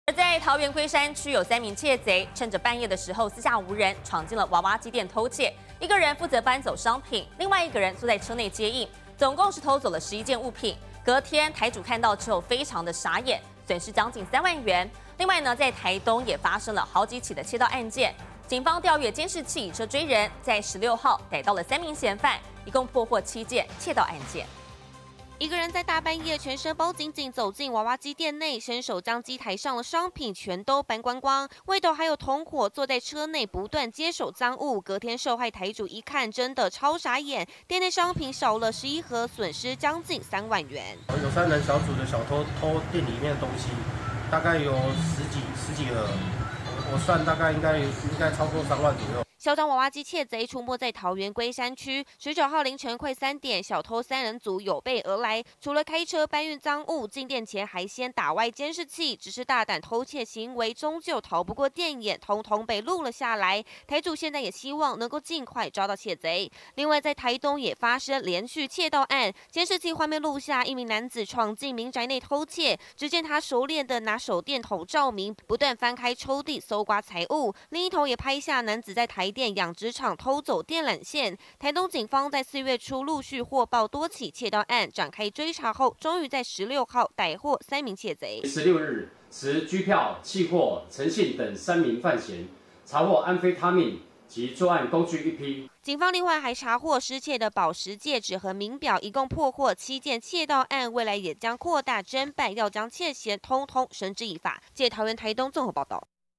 傻眼！11盒商品遭3賊搬上車 台主損失近3萬
昨天(19號)凌晨快3點，桃園龜山區有3名竊賊，趁著半夜四下無人，闖進娃娃機店偷竊，一人負責搬走商品，一人坐在車內接應，總共偷走11件物品，台主損失將近3萬元；另外台東最近也發生，多起竊盜案，警方調閱監視器，以車追人在16號逮到3名竊嫌，一共破獲7件竊盜案。